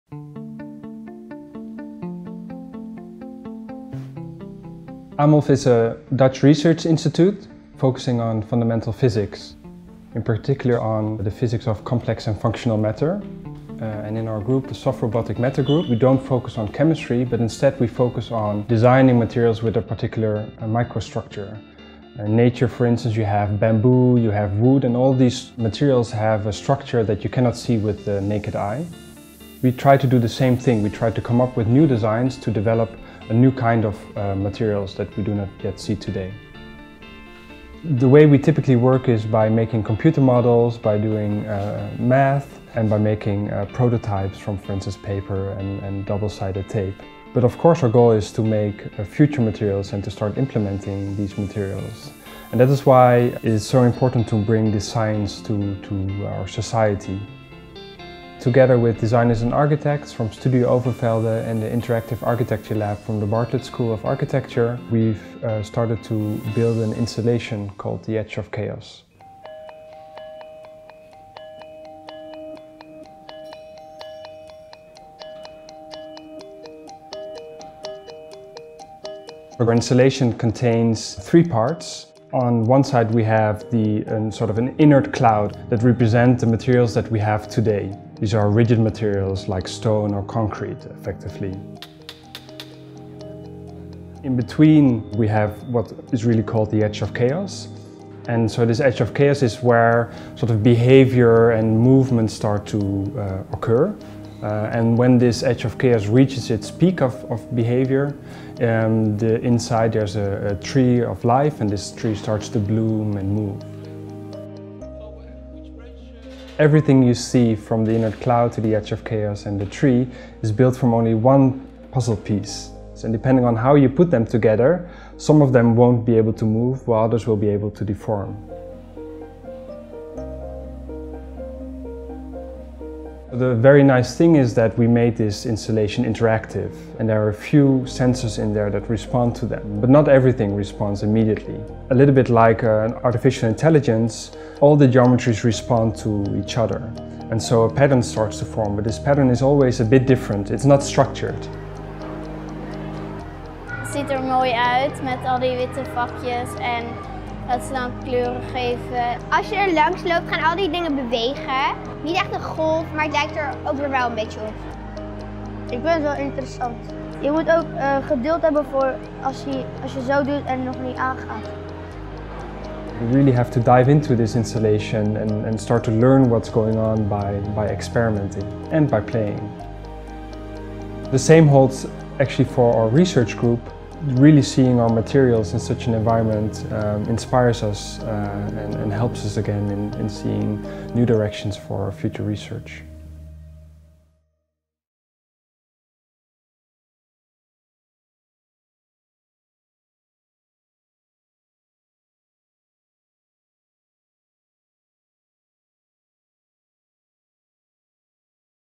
Science + Art = Kinetic Installation "Edge of Chaos"
Between order and chaos, a mathematical space originally studied to understand the behaviour of avalanches and crystallisation of liquids, scientists are uncovering the rules for the existence of life itself. A small region called the “Edge of Chaos” displays complex behaviour that is both organised and unstable, creating the continuous flux that scientist hypothesise drives the engine of life – evolution – and produces the boundless novelty of the natural world.

Inspired by the science of complexity, Edge of Chaos is a collaboration between scientist Bas Overvelde (AMOLF and Studio Overvelde) and artists Vasilija Abramovic & Ruairi Glynn (Interactive Architecture Lab, Bartlett UCL, bringing together the scientific work on transformable geometries previously published by Overvelde et al. in Nature and Interactive Architecture Lab’s expertise in immersive spatial design and interaction, the team collaborated on developing their competition winning entry for the international touring exhibition Capitaine Futur which opened in Paris at La Gaîté Lyrique on the 4th April.

Scientific advances in fabrication technologies are enabling the production of materials that get their properties from their structure, rather than only from their chemistry. They are man-made materials of the future, and exhibit properties that do not yet exist in nature. In the design of some materials it is possible to incorporate internal mechanisms capable of reconfiguring their spatial architecture, and in this way to enable tunable functionality. Inspired by the structural diversity and foldability of the prismatic geometries that can be constructed using origami techniques, Overvelde introduced a design strategy based on space-filling tessellations of polyhedra to create three-dimensional reconfigurable materials. The underlying principles are scale-independent, and can be applied to the design of the next generation of reconfigurable structures and materials, ranging from nanometre-scale systems to metrescale transformable architectures.

In a futuristic world expanding on this design strategy, the interactive installation “Edge of Chaos” invites visitors to experience the balancing point between highly ordered and turbulent systems, composed of three features. At its centre, a robotic tree representing “Life”, surrounded by an inert “Cloud” representing the vast unorganised matter of an entropic universe, and an interactive surface that represents the “Edge of Chaos”. The robotic tree and concept formation behind the installation was developed by the Interactive Architecture Lab designers, harnessing passive and active geometries that incorporate over 25.000 building block units designed and developed by Studio Overvelde. The interactive surface and its emergent behaviours were developed between the labs at AMOLF, Amsterdam, and Bartlett UCL, London celebrating creative possibilities of collaborating across science, art and design.

The narrative installation makes tangible the science of materials and complexity to children and adults alike, demonstrating its behaviour through sound, light and robotic motion, encouraging playful exploration of worlds to come. Just like Mort Weisinger’s space-traveling scientist and adventurer traveler: Capitaine Futur.

Edge of Chaos will be exhibited at La Gaîté Lyrique [Paris, FR], Cinekid Festival [Amsterdam, NL], KiKK Festival [Namur, BE] and WoeLab [Lomé, TG] in an international tour starting April 2018 and ending beginning 2019.

CREDITS
Artists: Vasilija Abramovic, Ruairi Glynn and Bas Overvelde
Interactive Architecture Lab, UCL [UK], AMOLF / Studio Overvelde [NL].
Co-produced by: La Gaîté Lyrique, KIKK Festival, Cinekid Festival and WOELAB.
Video: 5 Graden Oost
Sound design installation: Emmett Glynn.
STUDIO OVERVELDE Paris and Amsterdam team: Sanne Overvelde-Slagman.
AMOLF Paris and Amsterdam team: Dennis Kistemaker, Niels Commandeur, Giorgio Oliveri, Agustin Iniguez, Luuk van Laake, Davide Bray.
IALab Paris team: Naomi Li, Parker Heyl, Marguerite Tricaud, Ronan Glynn.
Special Thanks to: Dirk-Jan Spaanderman, Soft Robotic Matter Group, Enrico Cacciapuoti, Hui sim Chan, Marianna Chrapana, Parvin Farahzadeh, Buse Gurbuz, Dhruv Kumar, Alexandra Niaka, Isabella Ong, Pawimol Samsen, Oliver Townsend, Michael Wagner, Sana Yamaguchi, Min Zhang.